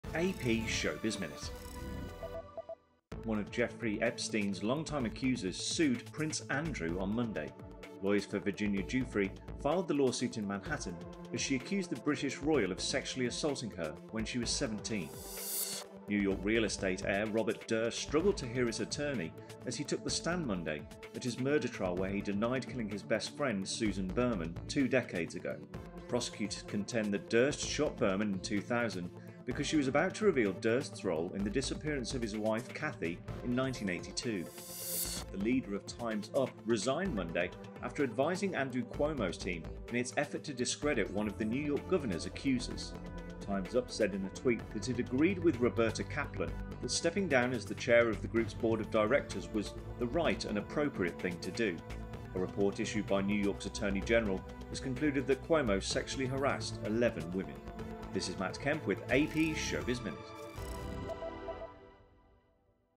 ShowBiz Minute: Prince Andrew, Durst, Cuomo
Epstein accuser sues Prince Andrew, citing sex assault at 17; Frail-looking Robert Durst denies killing best friend; Time's Up leader resigns after criticism about Cuomo ties. (Aug. 10)